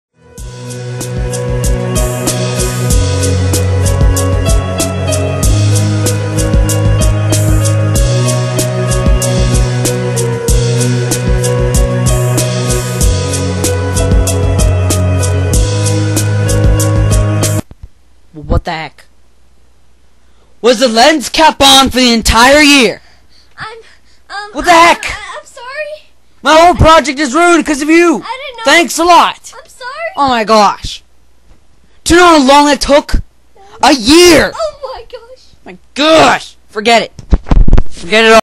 1 Picture Everyday For a Year Spoof
This is my spoof of all those video's of people takeing picture of themselves for years on end.